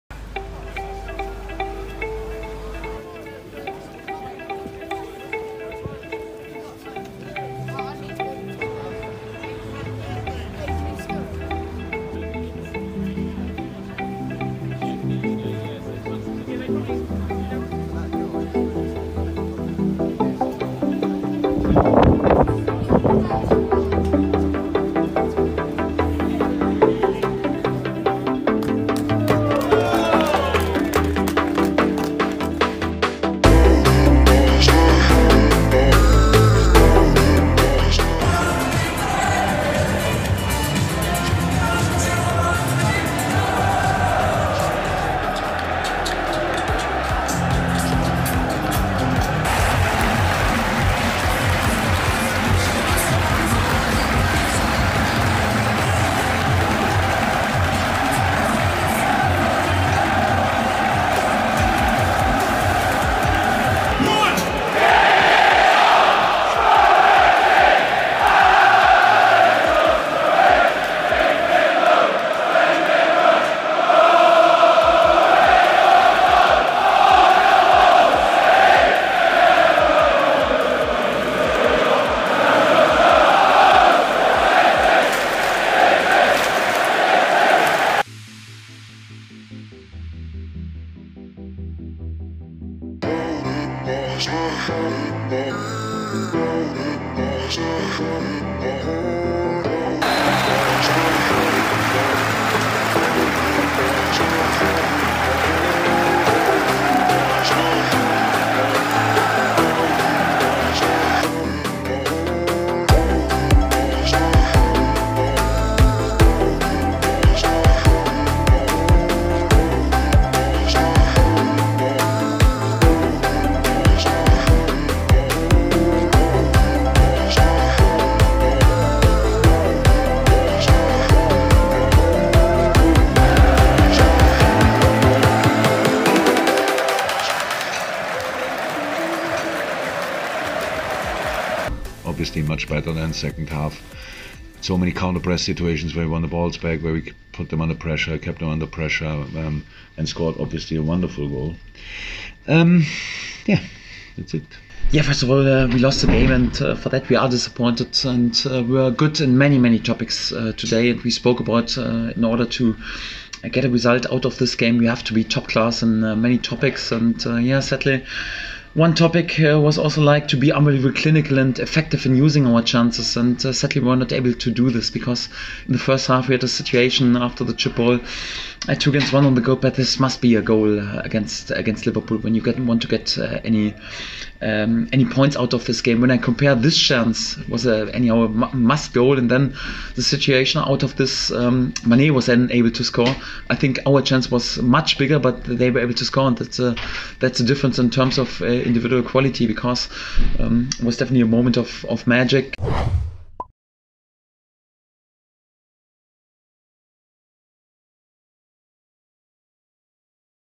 City Sights: Norwich City 0-1 Liverpool (Carrow Road)
Norwich City helf firm until the 78th minuted against Champions to be Liverpool at Carrow Road, watch and listen to all the best sights and sounds from around the ground in the Premier League.